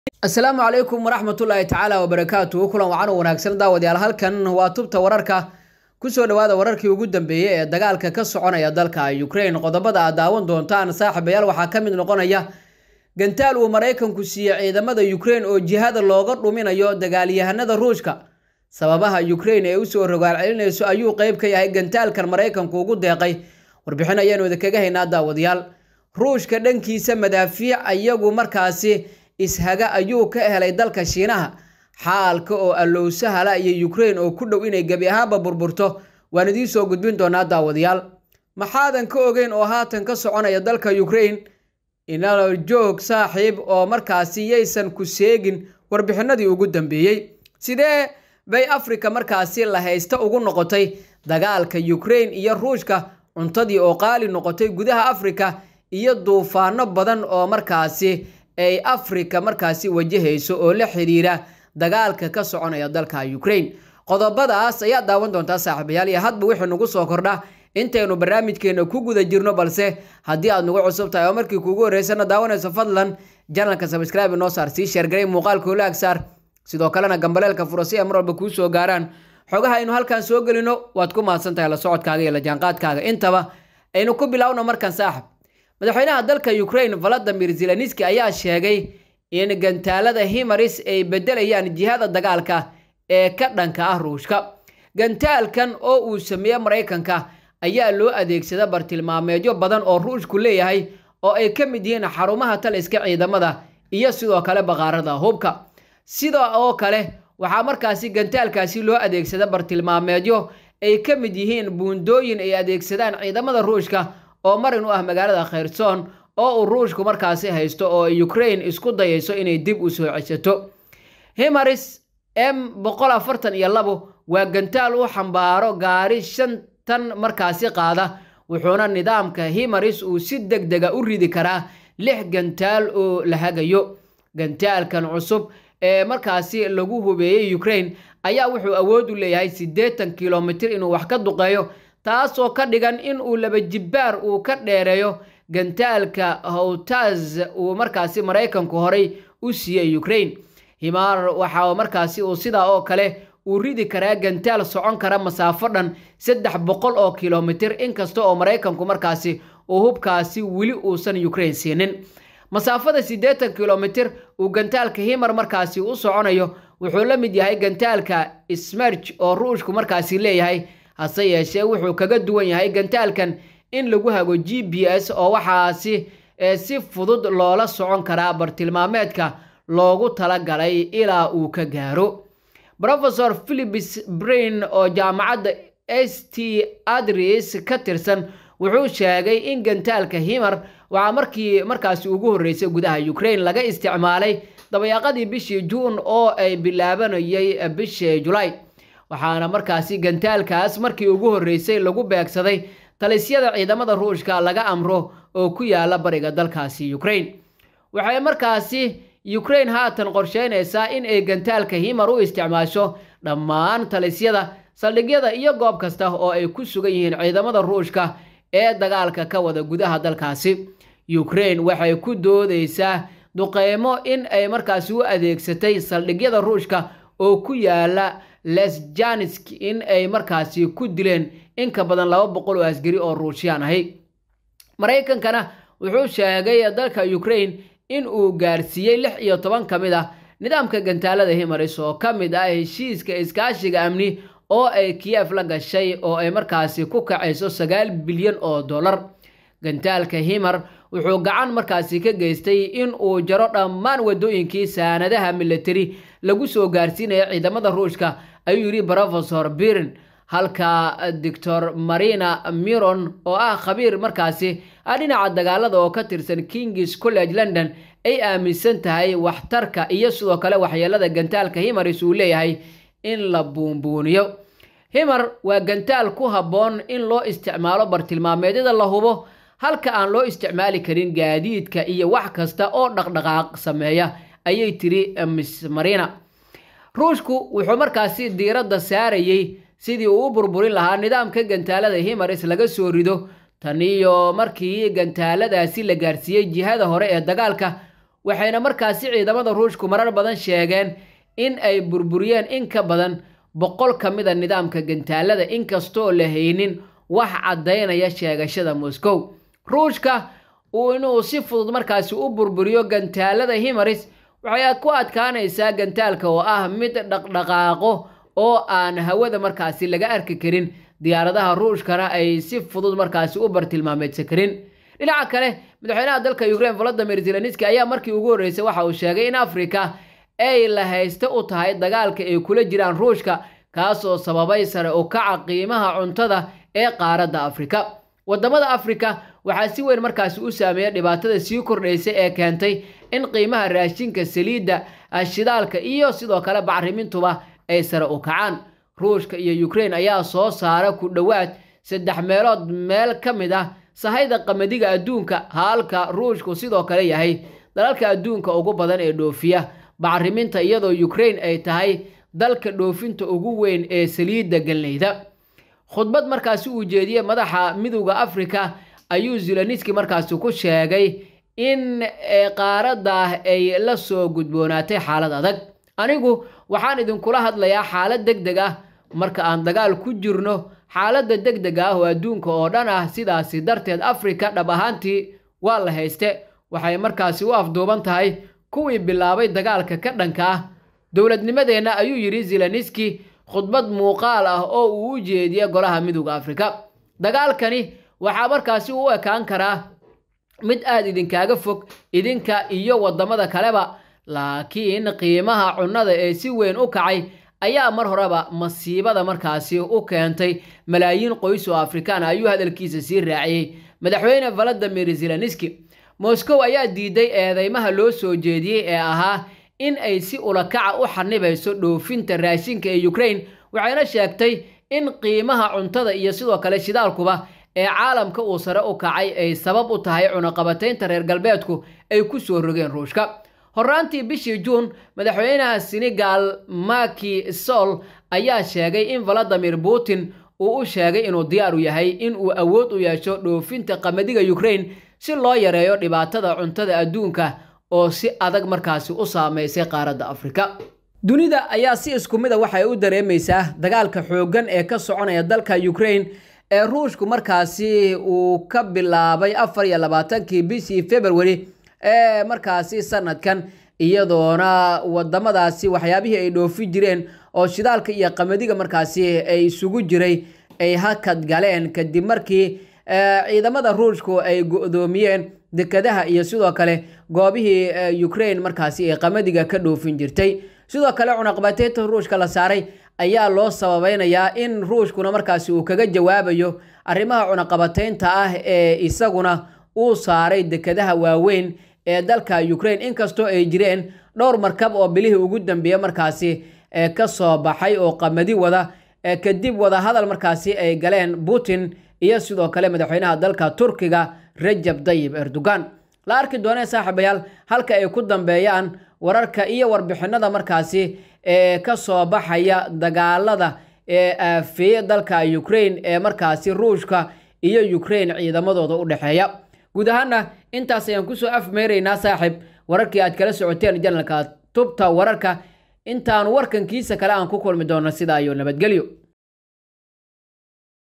WAR DEGDEG AH UKRAINE OO DIB USOO LAABATAY, CIIDAMADII RUUSHKA OO LAGA SOO SAARAY GUDAHA----
DEGDEG JARMALKA OO DAGAAL KU QAADAY RUUSHKA IYO PUTIN OO UKRAINE KASOO--